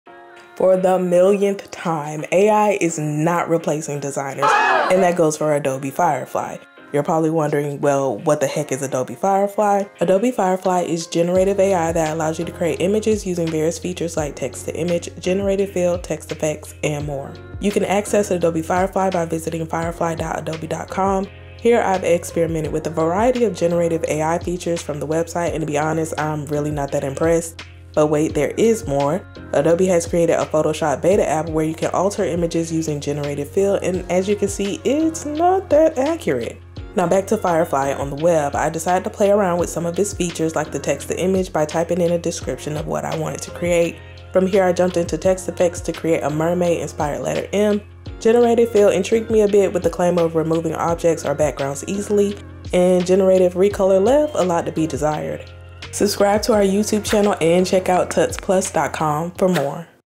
What Is Adobe Firefly?
AI is not replacing designers, and that goes for Adobe Firefly. What exactly is Adobe Firefly? Adobe Firefly is generative AI that allows you to create images using various features like text to image, generative fill, text effects, and more. ► Download Unlimited Stock Photos, Fonts & Templates with Envato Elements